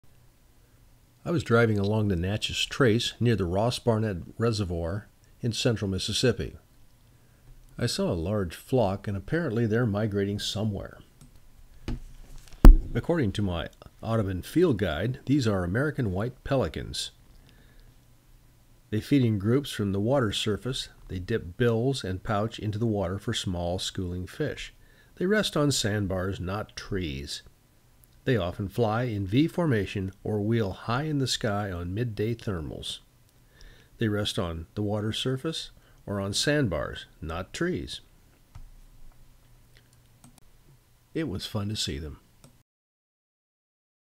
Pelicans migrate through Ross Barnett Reservoir in Mississippi
The annual migration of the American White Pelicans (Pelecanus erythrorhynchos) is an event area residents and bird watchers look forward to. The large white birds will arrive early in the year, in large flocks that may number in the thousands.